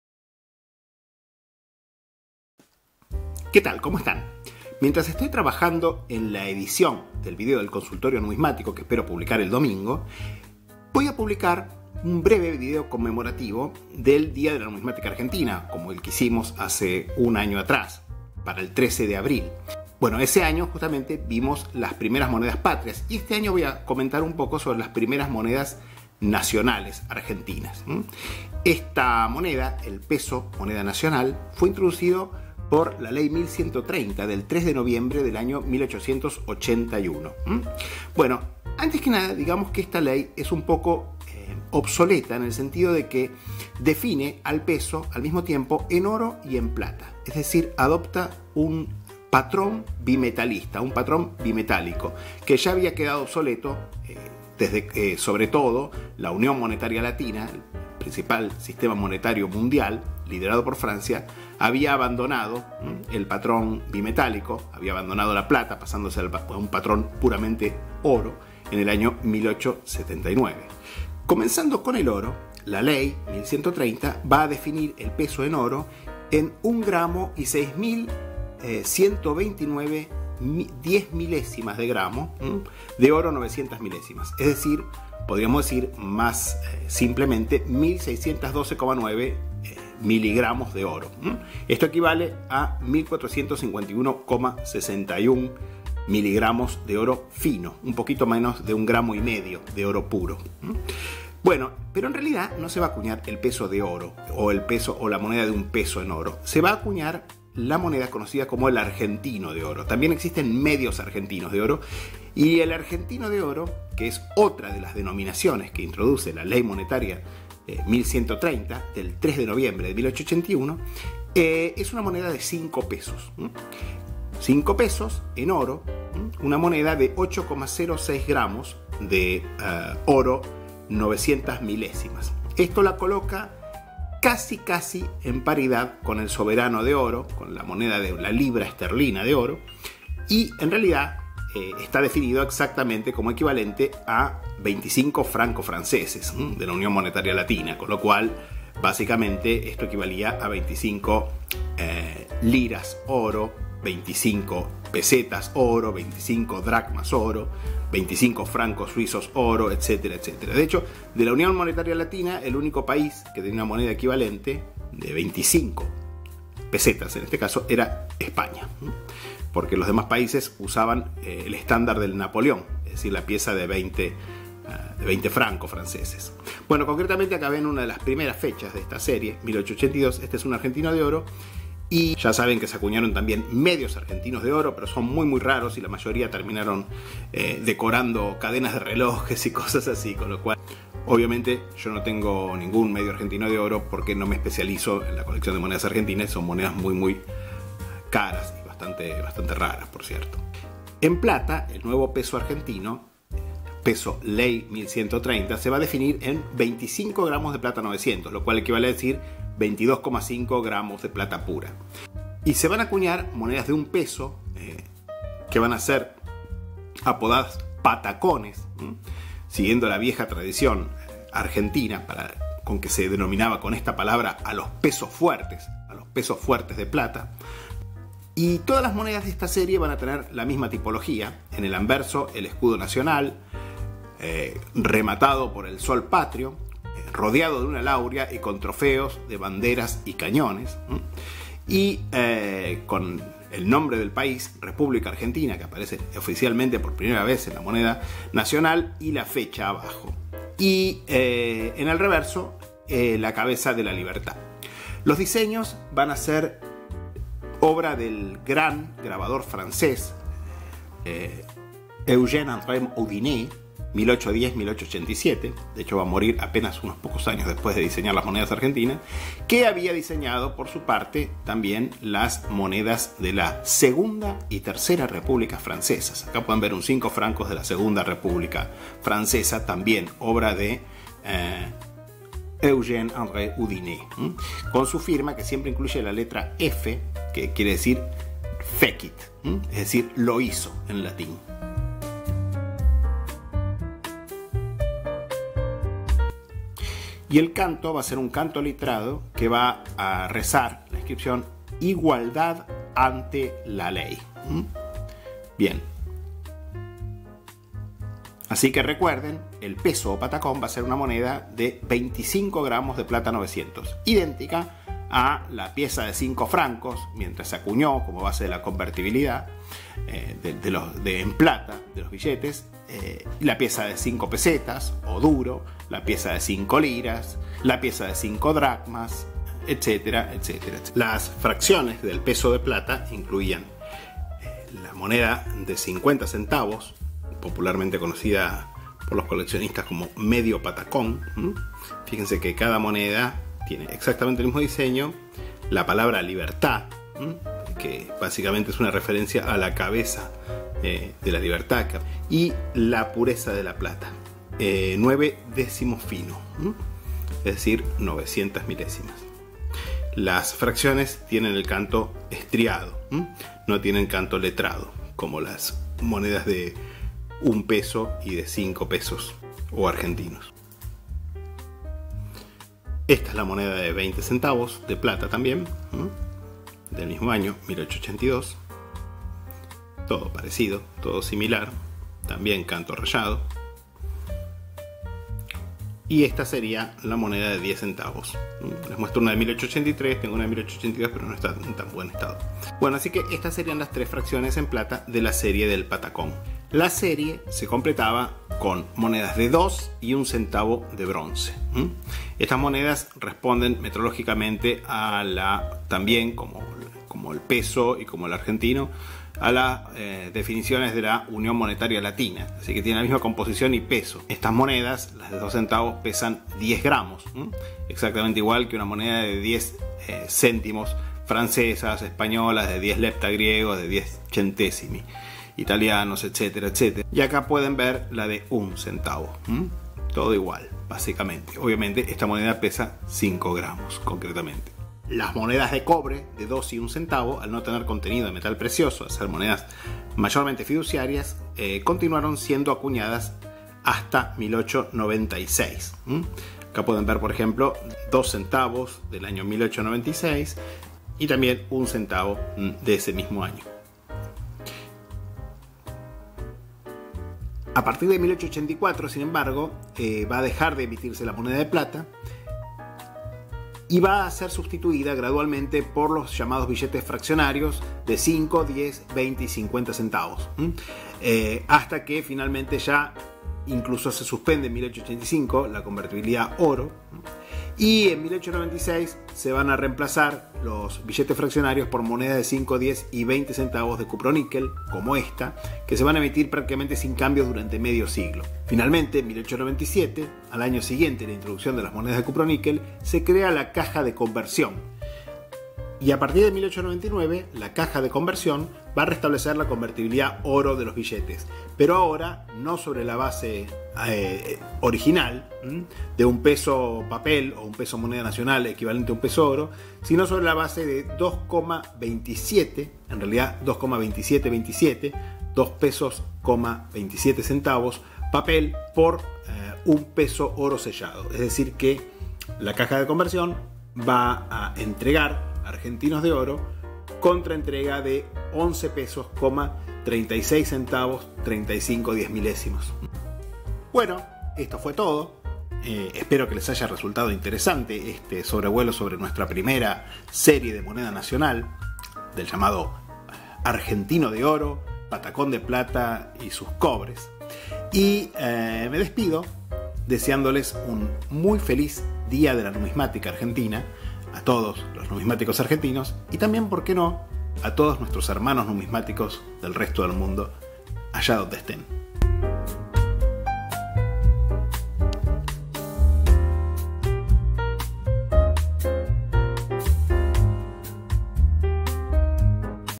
Argentino de oro, patacón de plata: primeras monedas nacionales [especial día de la numismática '22]
Nuevamente, y en conmemoración con el 13 de abril, DÍA DE LA NUMISMÁTICA ARGENTINA, presentamos un pequeño video, esta vez dedicado a la primera serie de moneda nacional, popularmente conocida como “serie del argentino de oro y del patacón”, que marca por primera vez la introducción del PATRÓN-ORO en la Argentina, y que incluye desde luego muchas monedas muy raras y valiosas, como el medio argentino de oro (1881, 1884) las monedas de 10, 20 y 50 centavos de 1881, y también algunas fechas de los “cobres” de dos y un centavo; sin embargo, casi todos los numismáticos argentinos tienen en sus colecciones al menos alguna de las monedas de estas series, ya sea las de 10 o 20 centavos de 1881 o 1882, ya sea alguno de los bronces, en especial los de 2 centavos, que (al menos en teoría) continuaron en circulación hasta fines de la década de 1940.